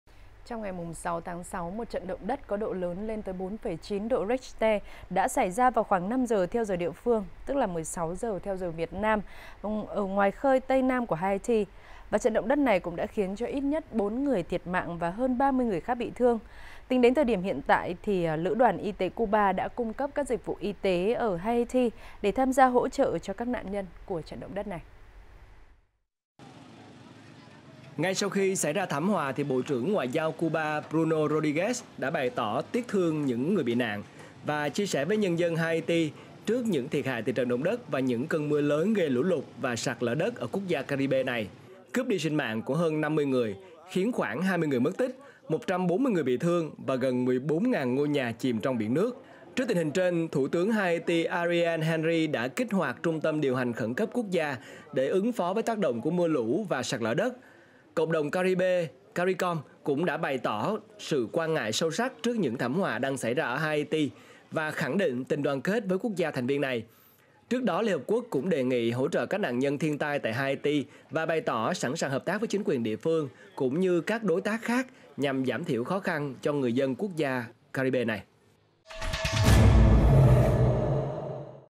HAITI: BÁC SĨ CUBA HỖ TRỢ CÁC N*N NHÂN TRONG TRẬN ĐỘ.NG ĐẤT | VTC9
VTC9 | HAITI: BÁC SĨ CUBA HỖ TRỢ CÁC N*N NHÂN TRONG TRẬN ĐỘ.NG ĐẤT
👉  Cập nhật nhanh tin tức văn hoá, xã hội, giải trí: TIKTOK/truyenhinhvtc9